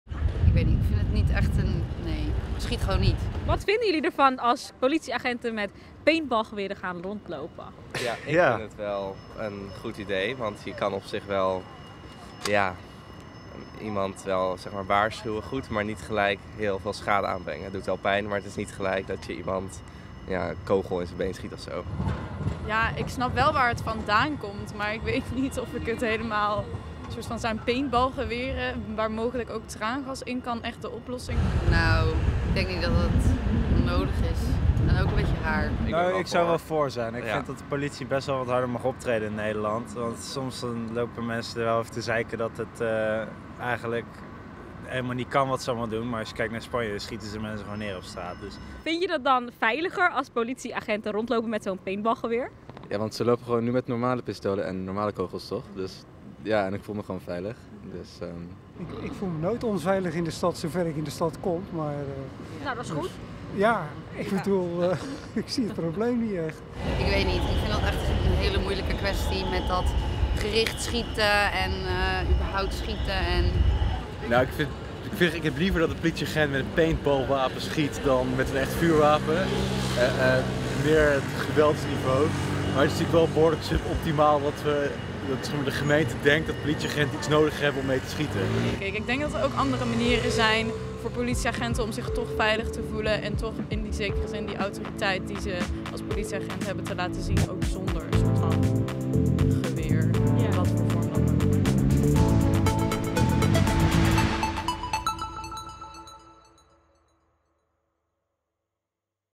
Burgemeester Aboutaleb wil politie uitrusten met paintballgeweer, goed idee? | OPEN Vraag
Als het aan Aboutaleb ligt wordt de Rotterdamse politie bij grote ongeregeldheden zoals de rellen op de Coolsingel in november vorig jaar, uitgerust met een paintballgeweer. De politie vraagt al langer om meer en betere geweldsmiddelen om grote rellen het hoofd te kunnen bieden. In andere landen gebruiken agenten al langer een paintballgeweer.